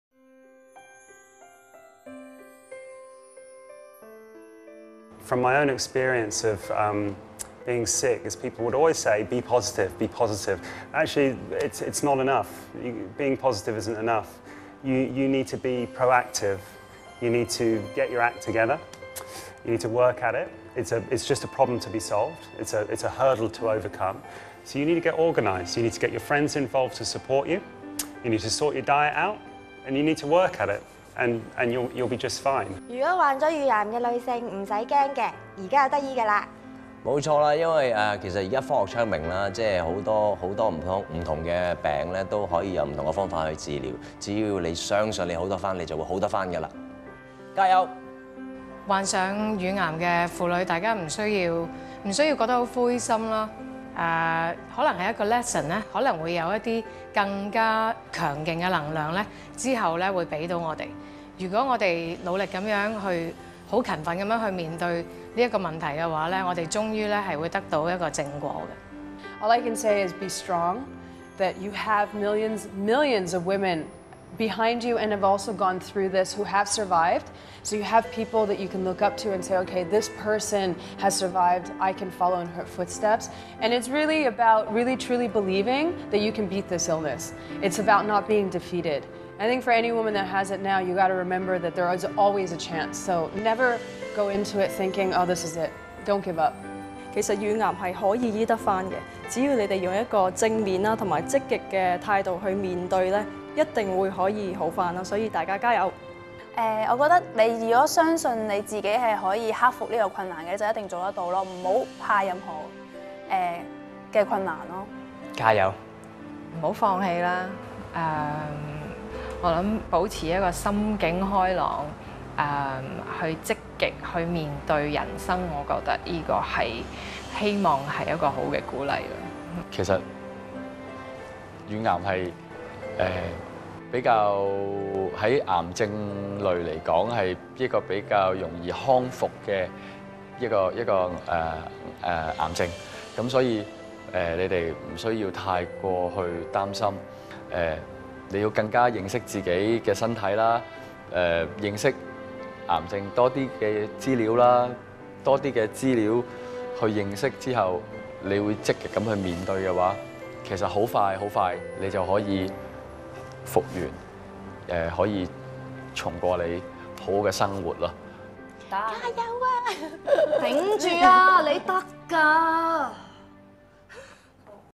Pink Revolution 2010 - Encouragement to women now living with breast cancer
Pink Revolution is Hong Kong Cancer Fund's annual breast cancer awareness and fundraising campaign in line with the global initiative for breast cancer. In addition to our push for awareness, we invite the community to help us raise HK$1.8 million towards our free and on-going breast cancer care services, as well as valuable research undertaken in Hong Kong.

Celebrity supporters including Josie Ho, Lisa S, Edmond Leung, Karen Lam, Sherry Tsai, Johanna Ho, Simon Birch, Phat Chan and more.